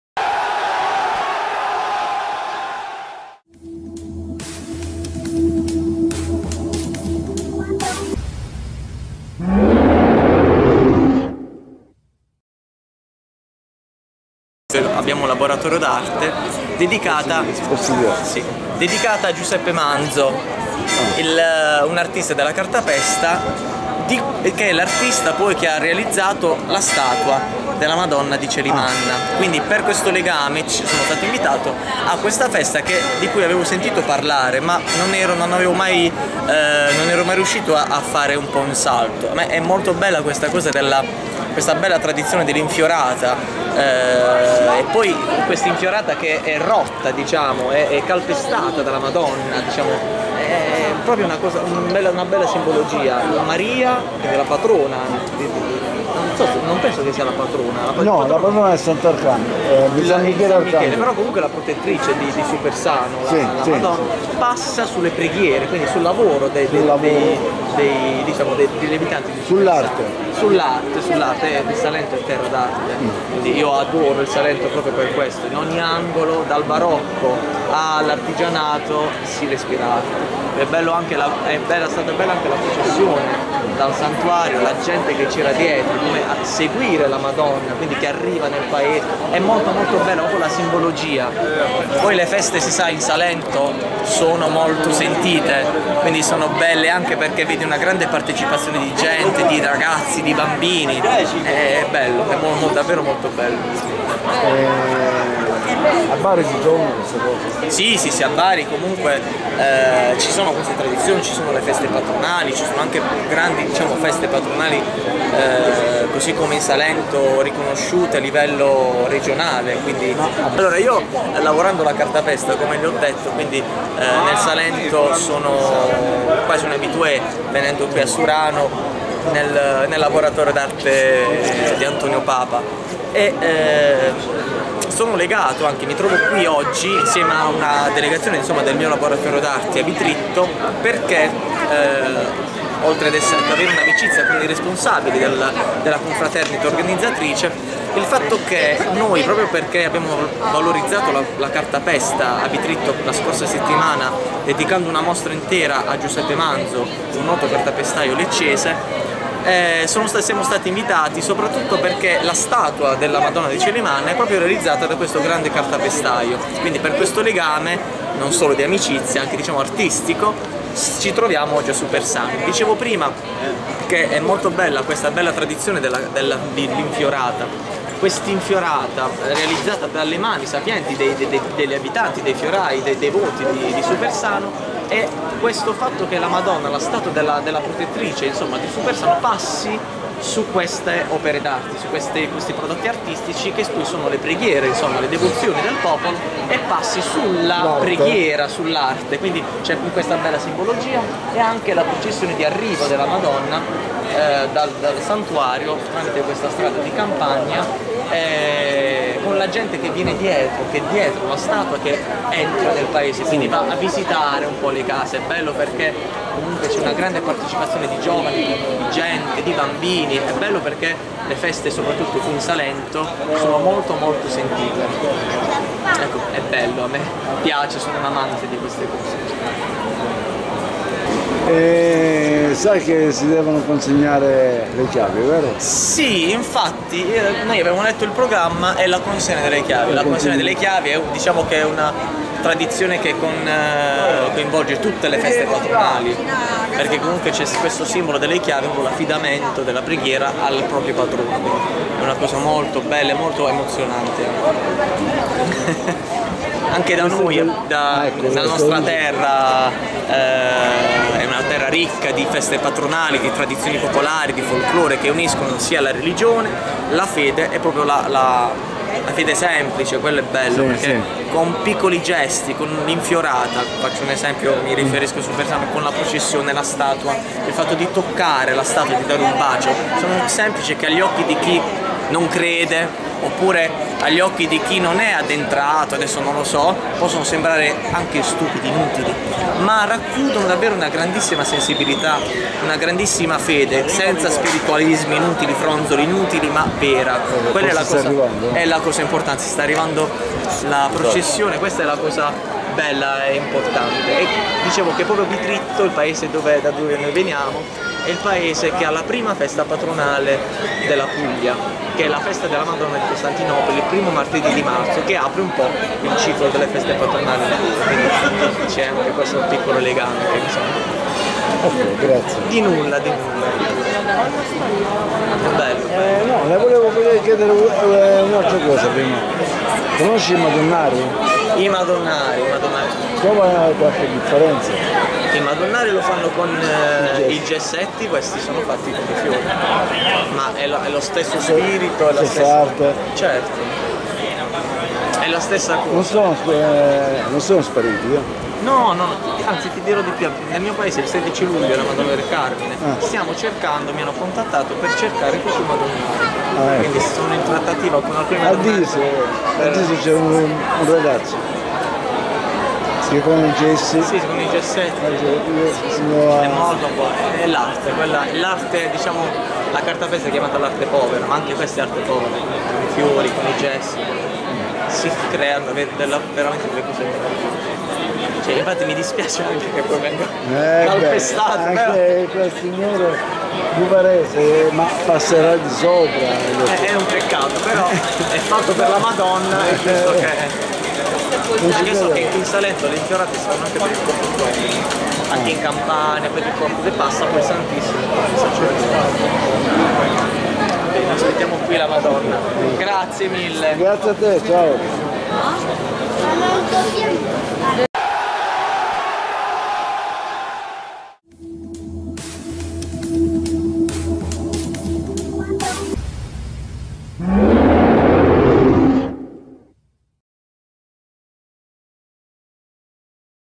Supersano Opinioni sulle "Infiorate " della Madonna del Coelimanna
Informarsi per informare

Supersano Persone di Bari manifestano gentilmente loro opinioni sulle "Infiorate " della Madonna del Coelimanna

monsellato project